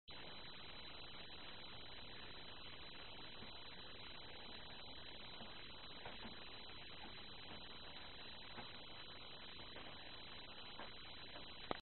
Tinkerbell
this is another kitty video of the rescued cats living in my shed.  Tinkerbell is an adorable kitten, who loves to wrestle with string toys